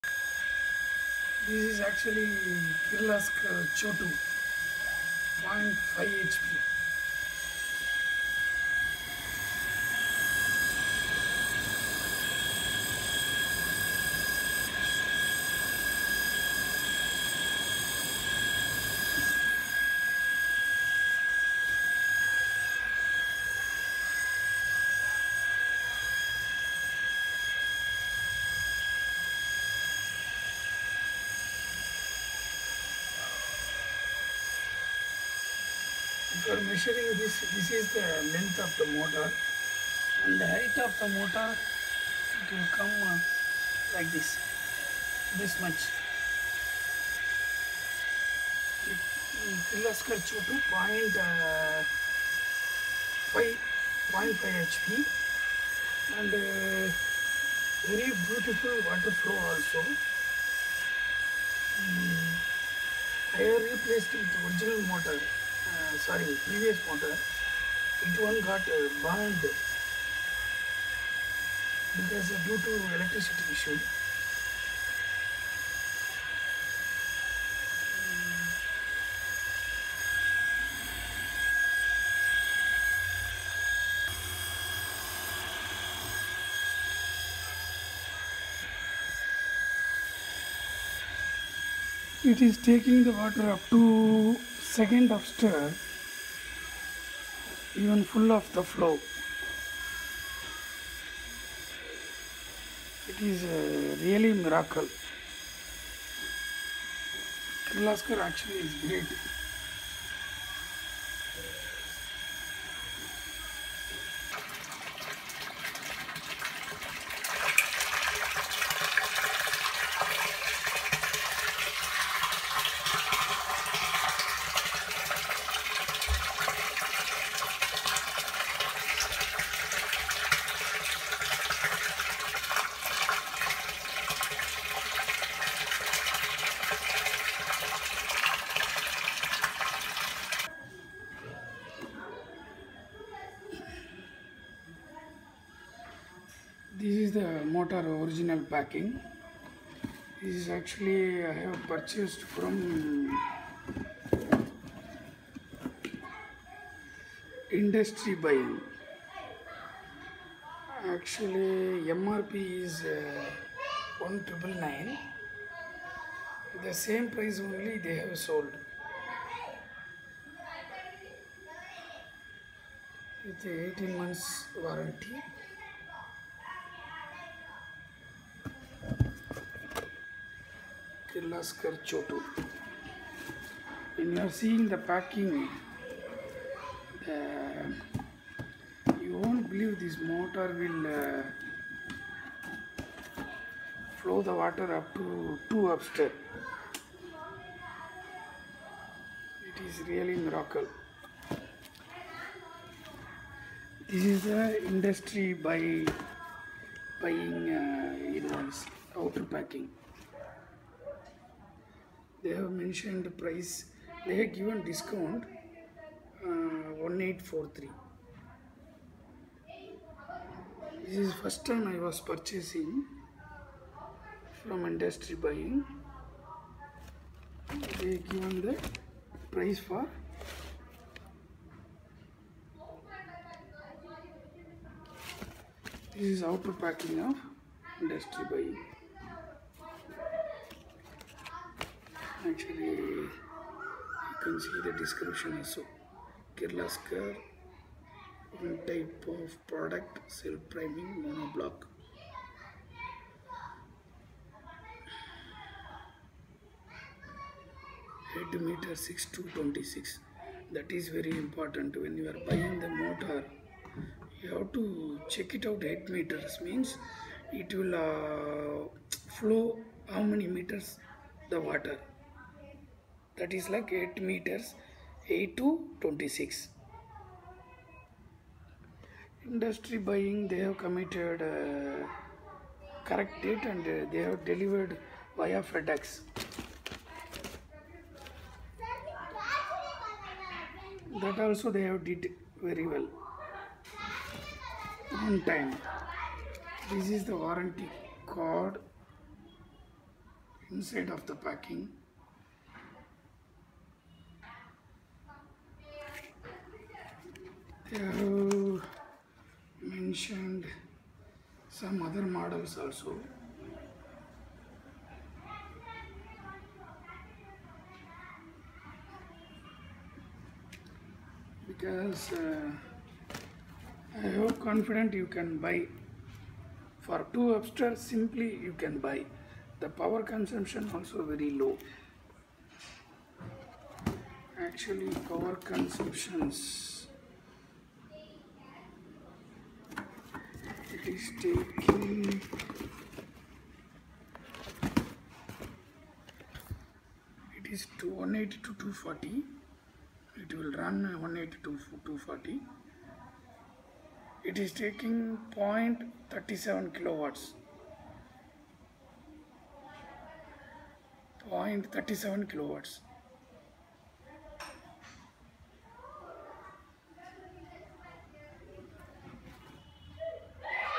Kirloskar 0.5 hp Mini Domestic Chhotu Pump (water Pump for home )
Special Motor design can work on UPS
It is capable of priming up to 3 meters static suction lift without a foot valve , at rated head & discharge
‘B’ class of Insulation.
Motor can withstand with wide voltage fluctuation from 180-

240Volts.
Light weight pump for easy handling
Capacitor inside terminal box.
TEFC capacitor starts and run motor
Water supply for domestic use in Bungalows, Apartments, Hotels, Farmhouses etc.
Useful in home pressure booster systems, lawn sprinklers, overhead tanks and watering for gardens.
Technical Specifications
Head : 6 to 26 meters
Capacity : 1980 to 360 LPH
Power Rating : 0.37 Kw / 0.5 HP
Voltage Range : 180 to 240 Volts (1Phase)
Material of Construction
Impeller : Brass
Pump shaft : Carbon steel
Delivery Casing : Cast Iron
Mounting casing : Cast Iron
Motor Body : Extruded Aluminum
Cover NDE : Aluminum
Mechanical seal : Carbon ceramic